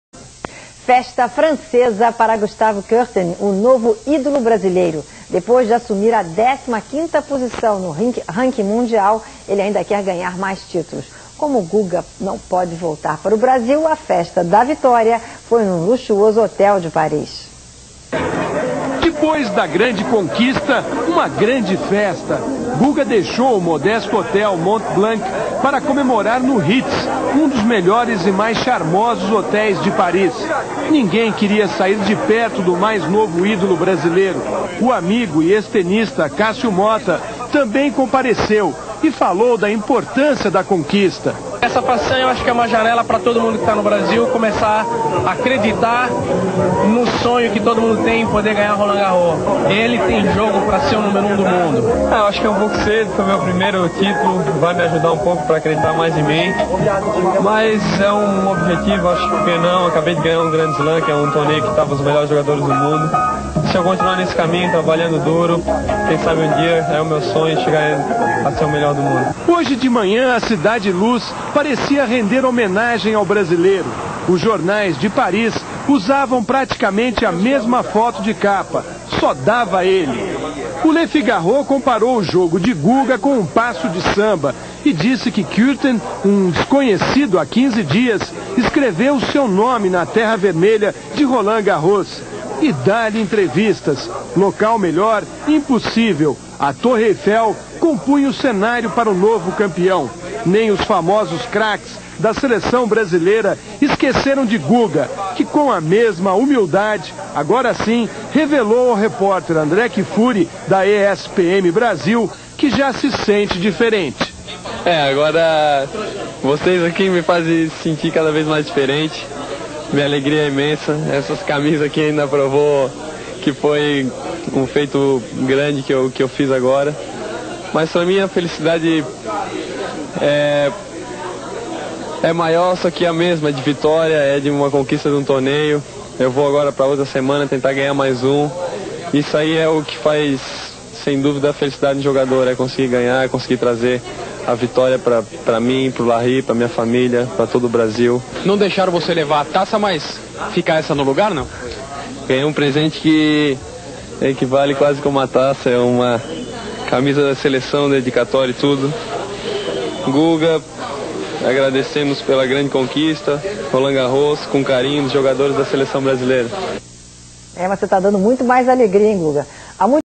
Gustavo Kuerten vence torneio de Roland Garros
Festa francesa para Gustavo Kuerten, o novo ídolo brasileiro. Depois de assumir a 15ª posição no ranking mundial, ele ainda quer ganhar mais títulos. Como Guga não podia voltar para o Brasil, a festa da vitória foi num luxuoso hotel de Paris.